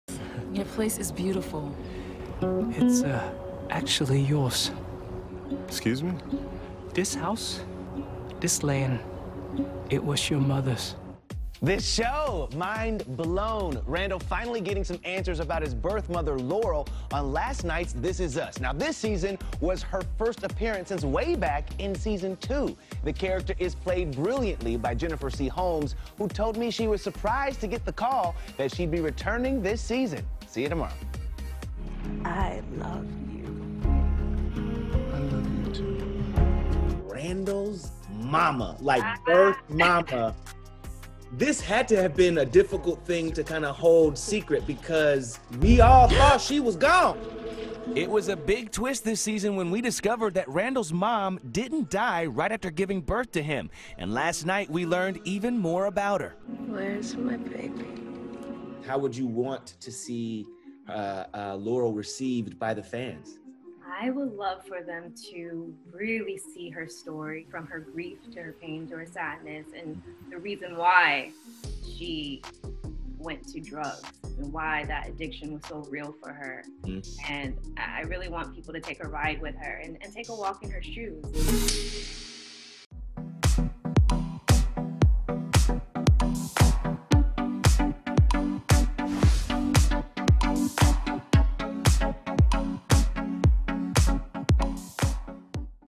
'This Is Us': Jennifer C. Holmes Talks Shocking Return
Jennifer C. Holmes is back on "This Is Us." Fans of the hit NBC series were finally able to learn what really happened to Sterling K. Brown's character's biological mom when Jennifer's character made a shocking return. Jennifer chatted with Access Hollywood's Scott Evans about the major plot twist and revealed what she hopes fans will soon find out about Laurel.